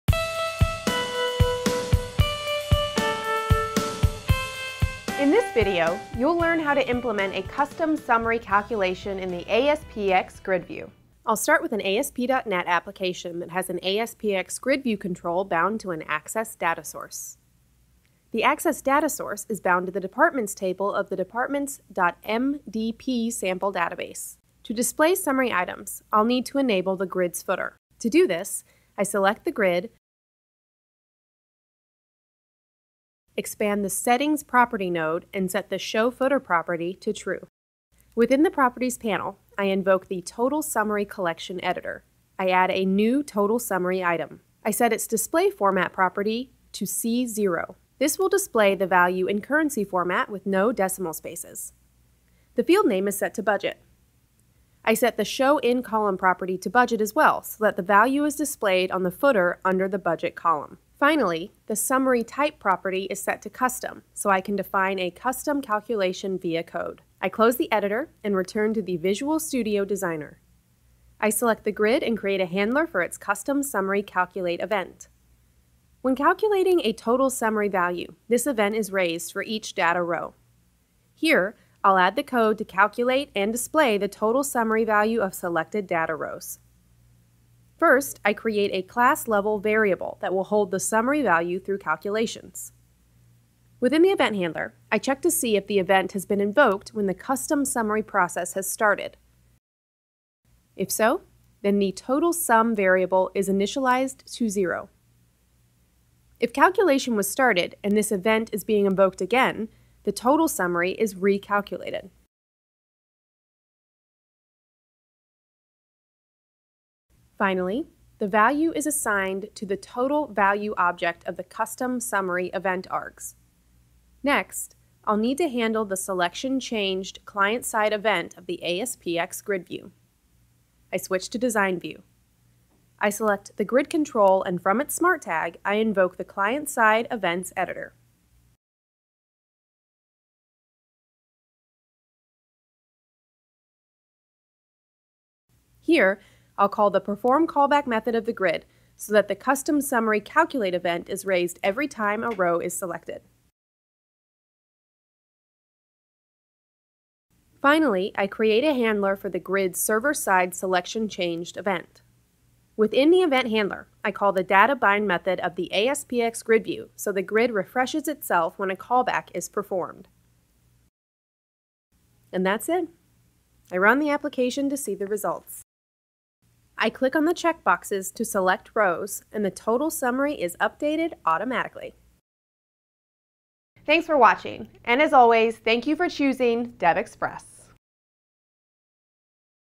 ASP.NET Grid - How to Calculate Custom Total Summaries
In this video you will learn how to implement a custom summary calculation in the ASPxGridView.